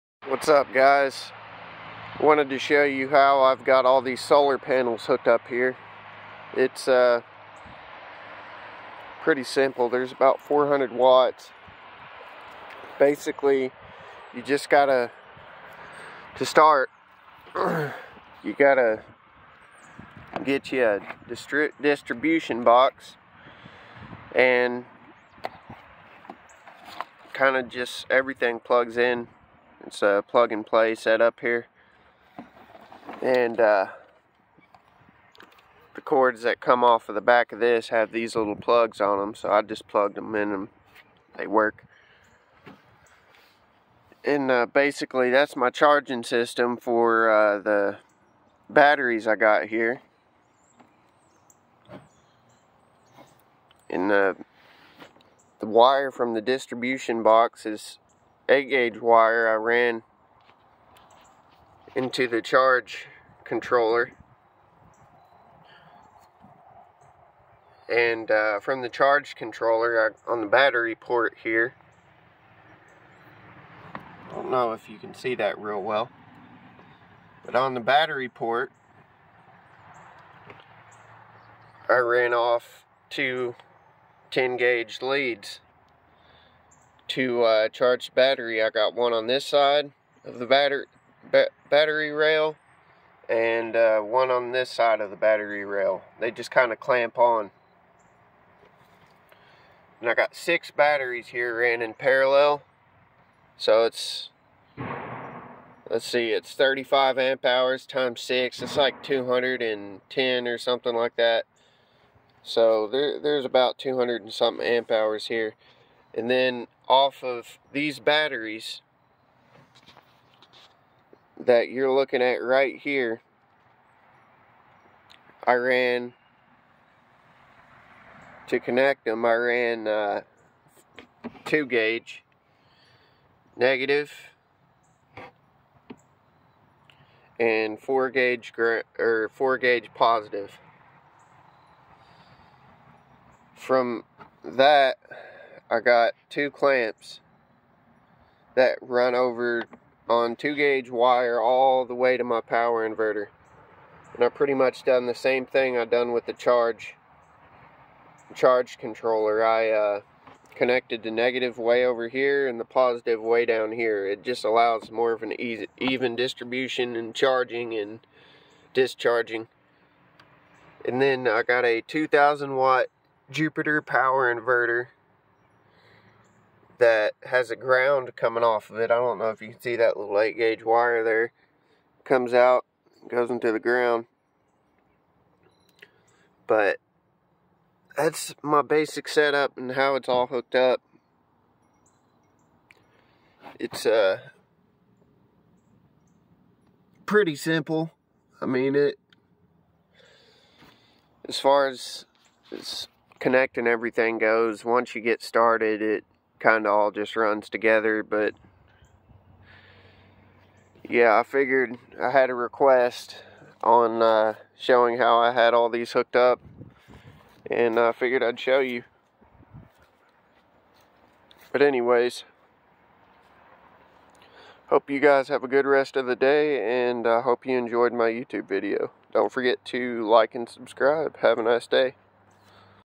How My Solar Panel setup is wired!!
This is just a description of how my solar panels are wired to the batteries for charging. The biggest thing is make sure you get the right size wire and you can never go to big. Hope y'all enjoy the video!